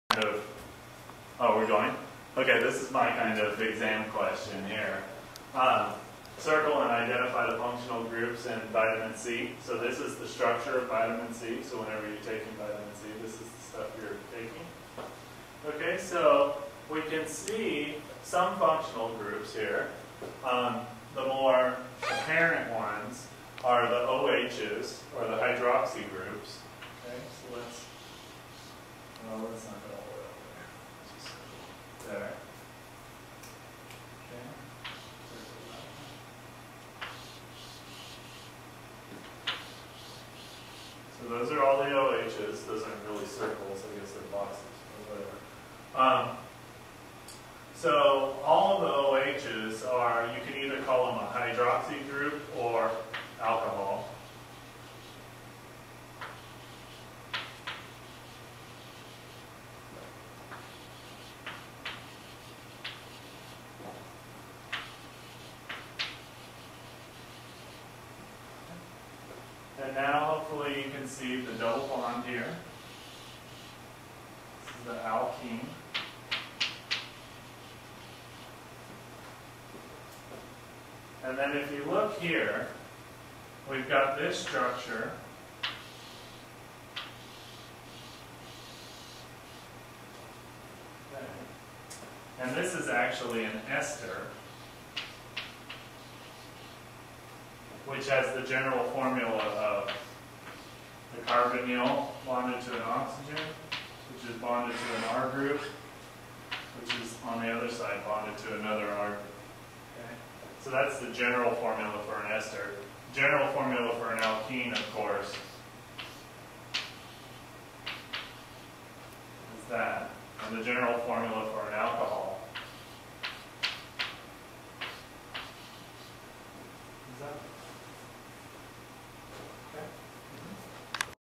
Circle and Identify Functional Groups In an Organic Molecule (Vitamin C) 001
Circle and identify the functional groups found in ascorbic acid (Vitamin C).

Interviews
1) Revell, K. (November 16, 2016) “An Interview with Heath Giesbrecht, Part I” The Macmillan Community

2) Revell, K. (November 16, 2016) “An Interview with Heath Giesbrecht, Part II” The Macmillan Community